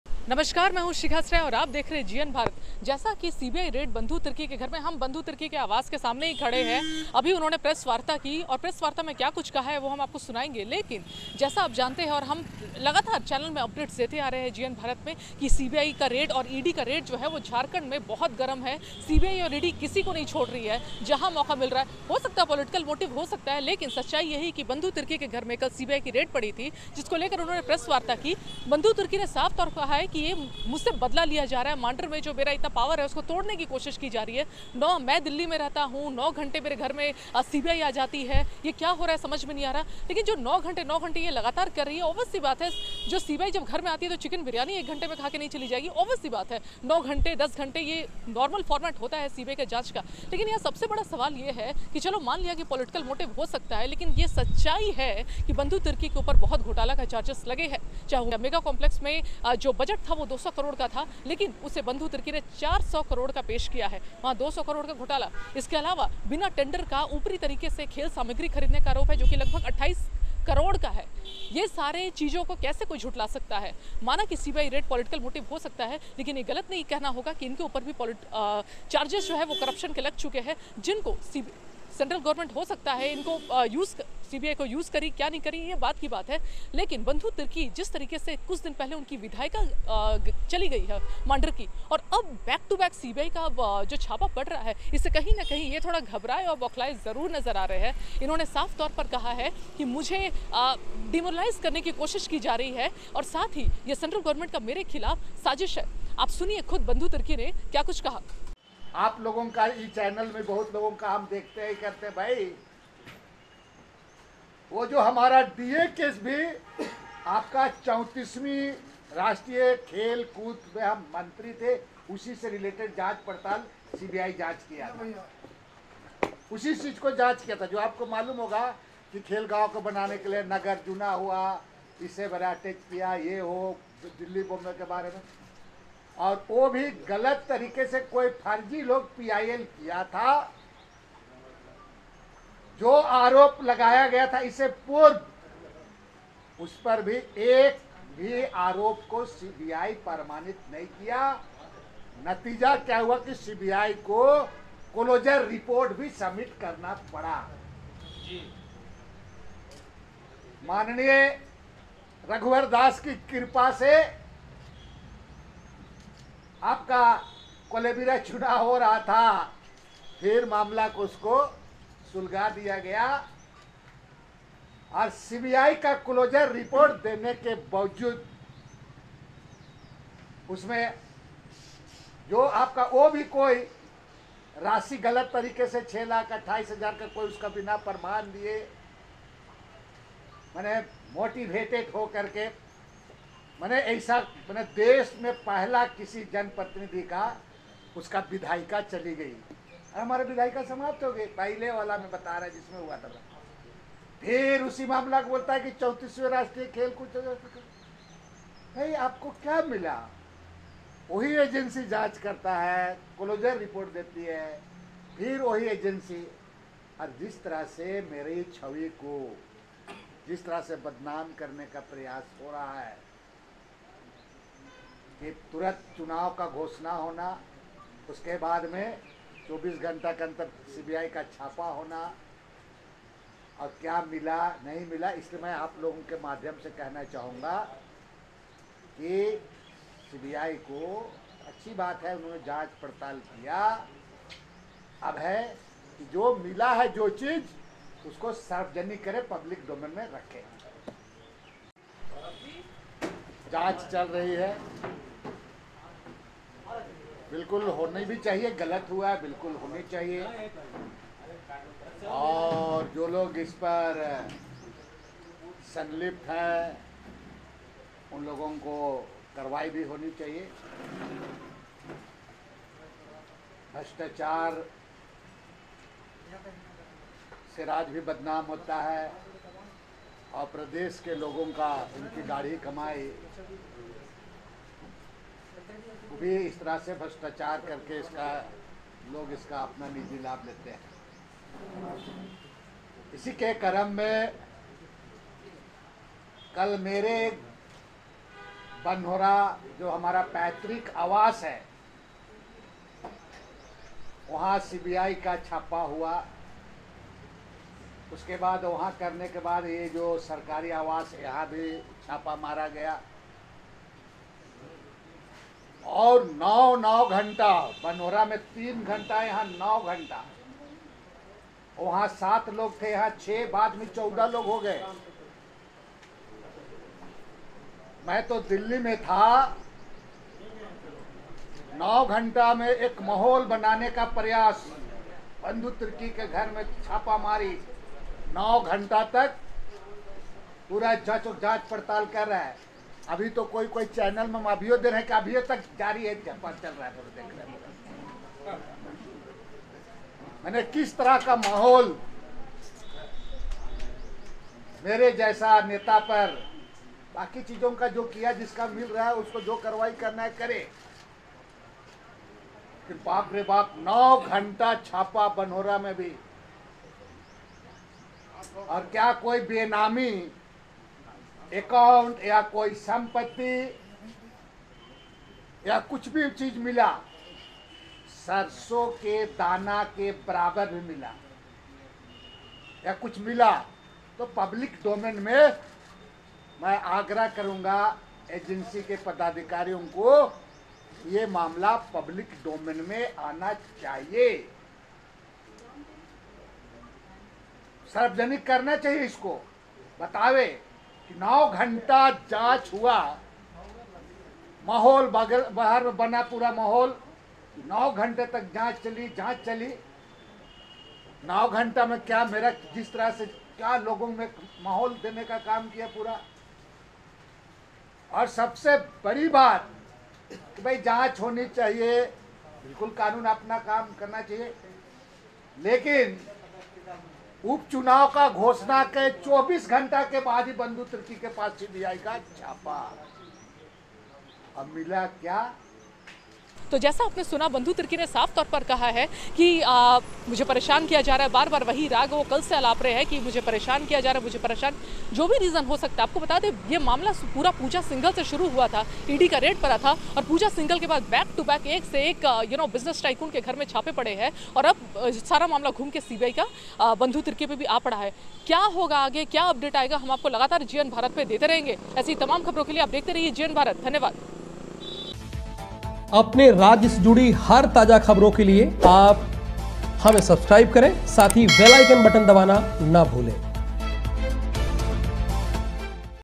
बंधु तिर्की के ठिकानों पर CBI का फिर छापा, प्रेस कॉन्फ्रेंस कर कह दी बड़ी बात
बंधु_तिर्की के ठिकानों पर CBI का फिर छापा, प्रेस कॉन्फ्रेंस कर कह दी बड़ी बात CBI_Raid #